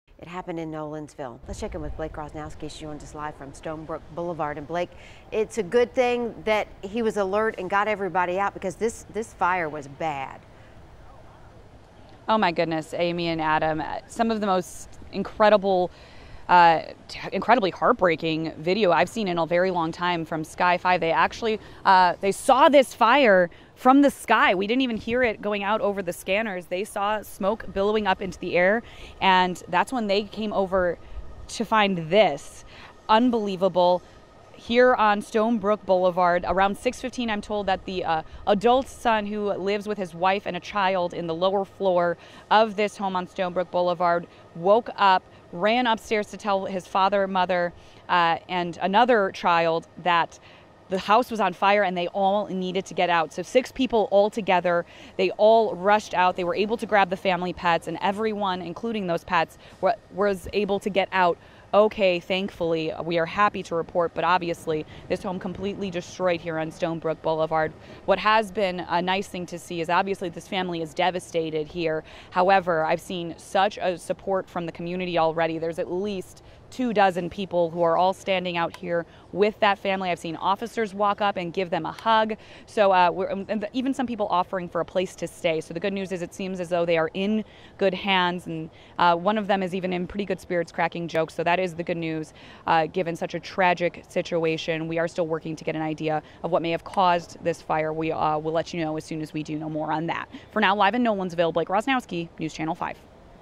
Family Escapes House Fire In Nolensville
Several people, including two children, were able to escape their burning home when fire broke out early Friday morning.